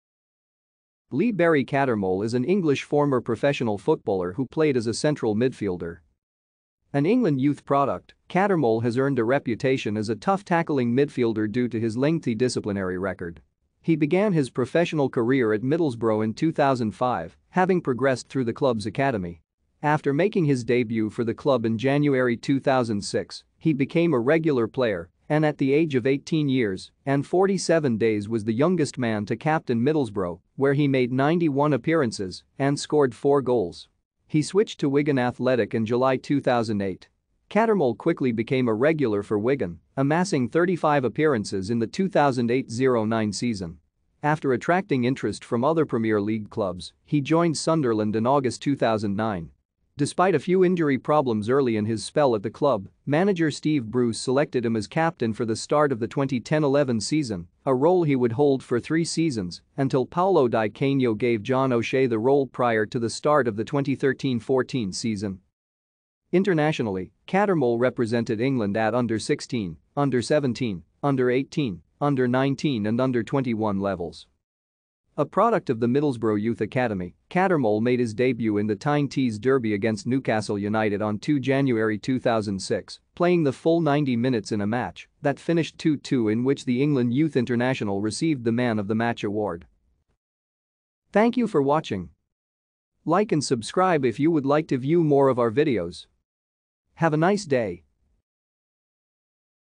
Lee Cattermole
Lee Barry Cattermole   is an English former professional  who played as a central midfielder.